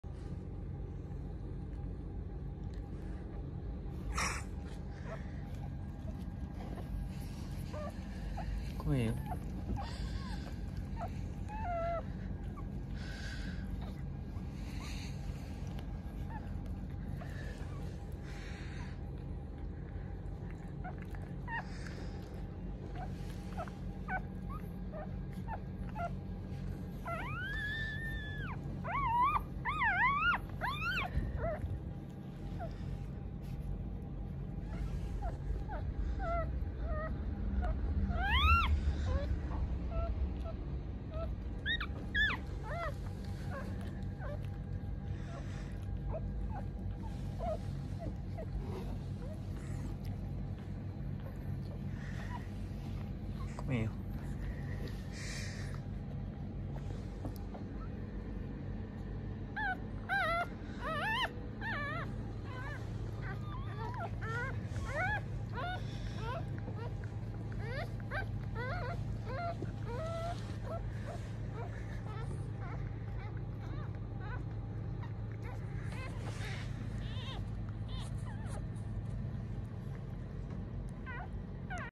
豆柴ちゃん三兄弟産まれました🥰🥰
12/19豆柴ちゃん3頭　♂2♀1出産致しました。🥰
カラーは赤2の黒1です
お母さんは出産はとても慣れていて上手で大きな事もなく無事に産まれました🥰
これからの子犬の成長過程上げていこうと思いますので是非チェック宜しくお願い致します🤲